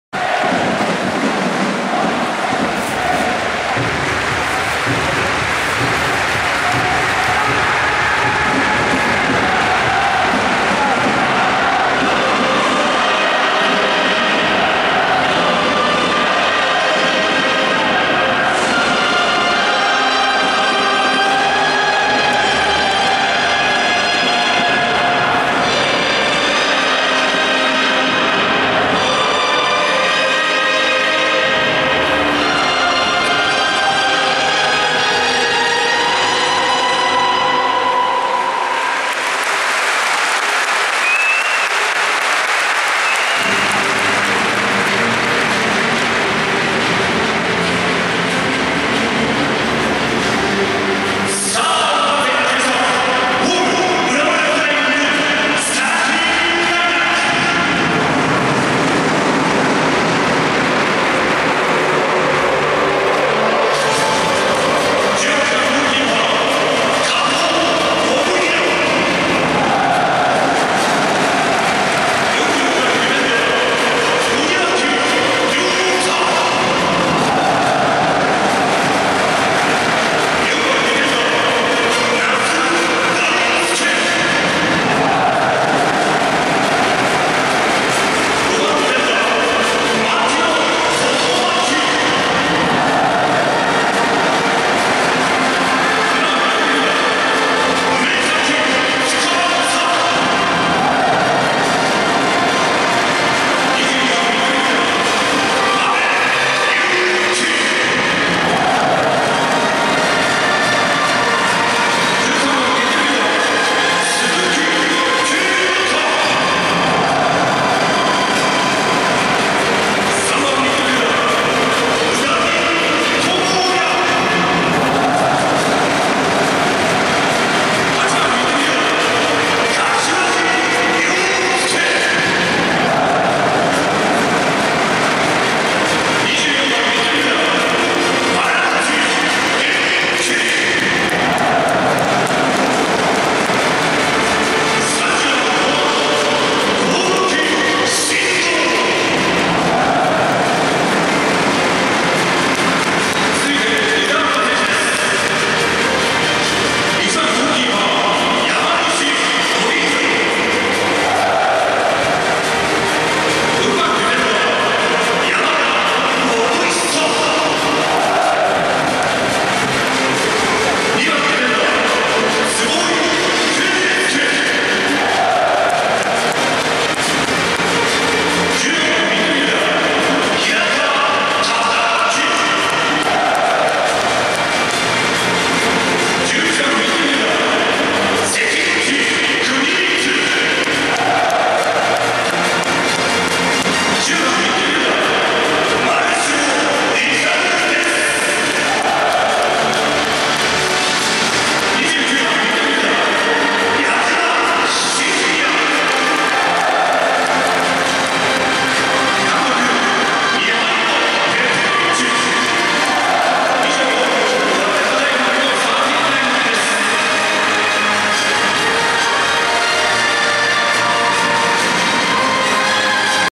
浦和レッズ 2013第5節vs磐田 選手紹介 2004メモリアル映像
2013.4.6　Ｊリーグ第5節　浦和レッズvsジュビロ磐田　埼玉スタジアム2002　試合開始前選手紹介　2004メモリアル映像